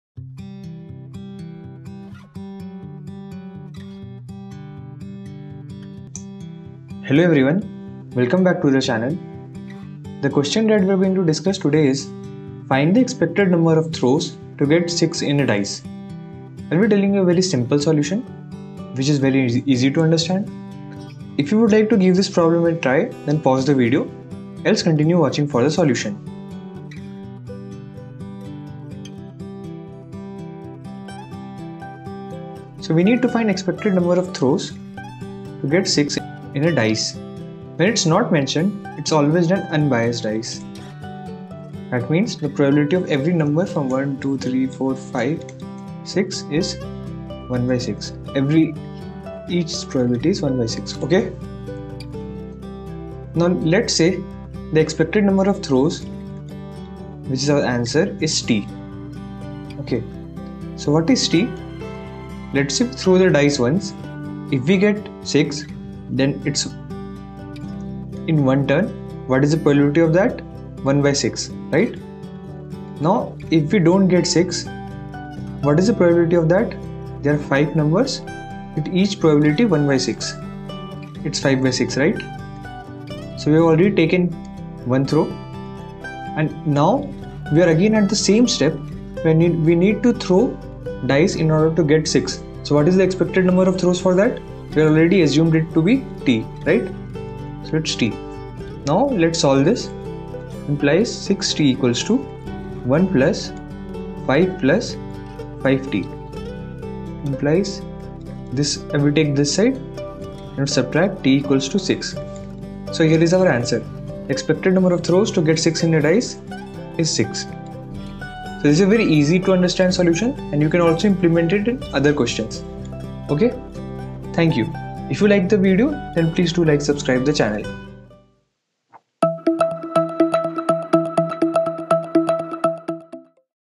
Expected number of throws to get 6 in a dice
What is the expected number of throws to get 6 in a dice?

For any queries related to the solution please feel free to drop a comment.